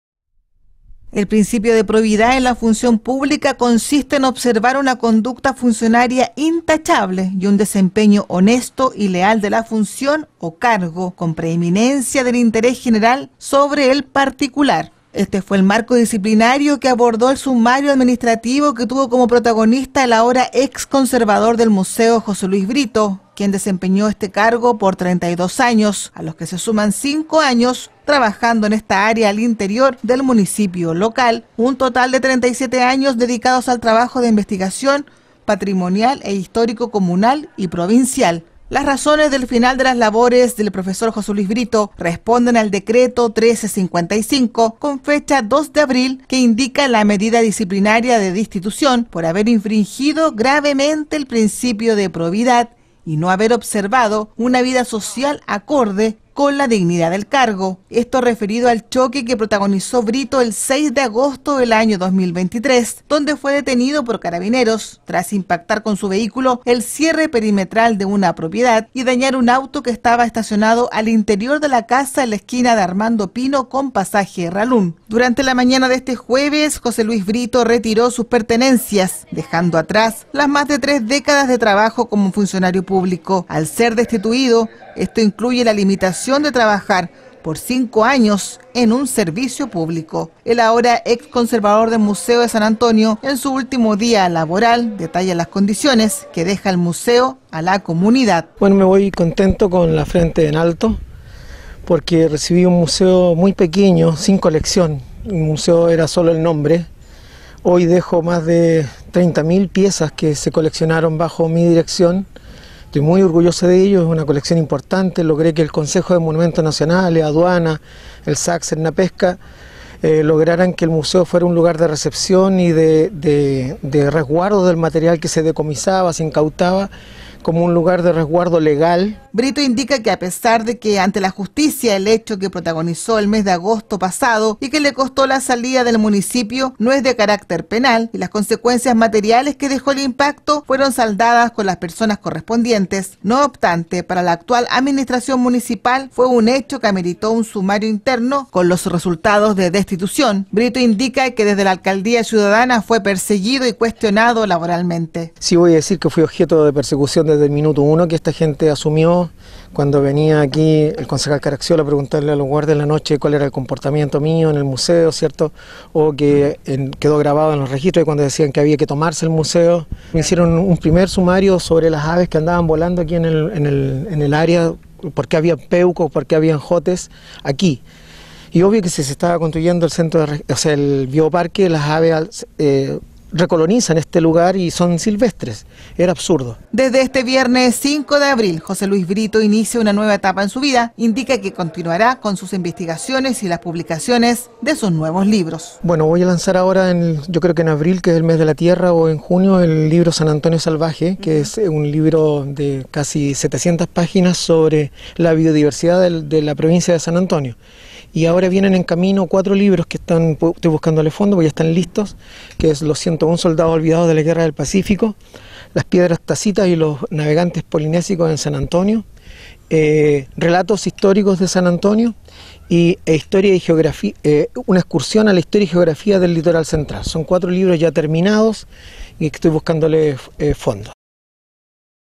04 ABR 2024 José Luis Brito: ¡Adiós! al Museo de San Antonio
Después de 37 años trabajando en el municipio de San Antonio, el ahora ex conservador del museo, deja su cargo por decreto resultado de un sumario administrativo, que indica destitución a causa de haber infringido el principio de probidad. Más detalles en la siguiente nota